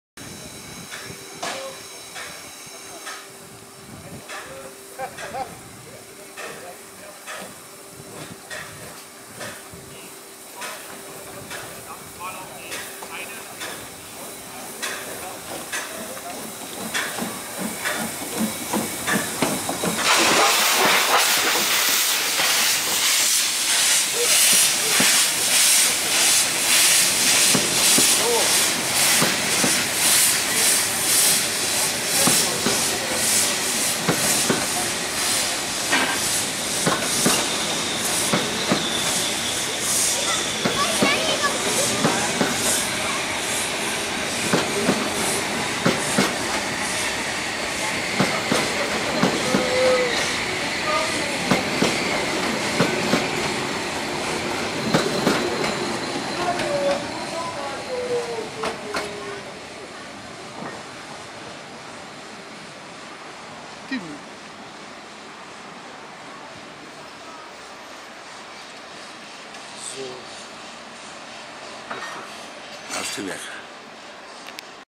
07-14.08.12 Privébez HSB (1328)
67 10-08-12 Bahnhof Wernigerode HSB De 99 7237 vertrekt naar de Brocken (01.14)